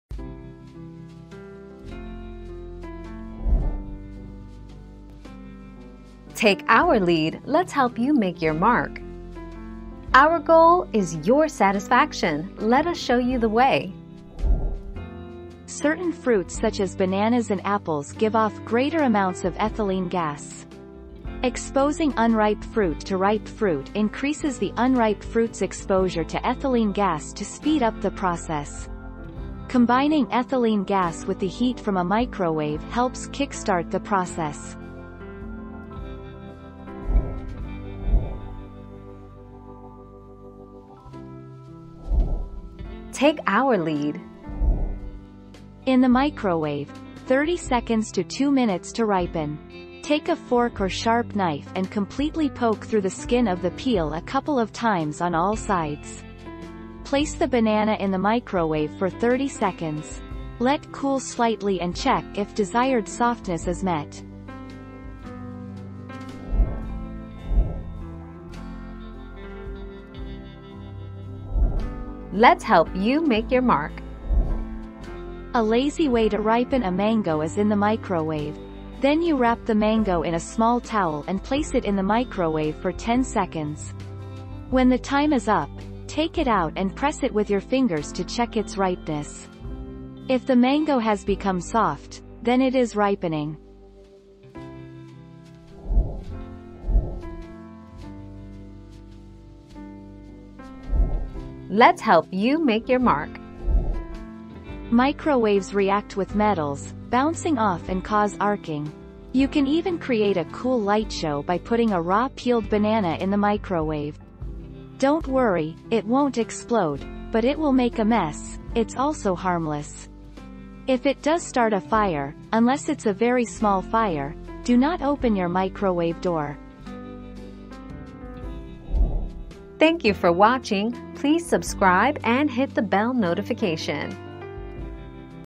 Can you ripen fruit in the microwave?
00:00 - Can you ripen fruit in the microwave?
00:39 - Can you ripen bananas in the microwave?
01:08 - Can I ripen a mango in the microwave?
01:37 - Is it safe to microwave a banana?

Laura S. Harris (2021, April 7.) Can you ripen fruit in the microwave?
    AskAbout.video/articles/Can-you-ripen-fruit-in-the-microwave-209807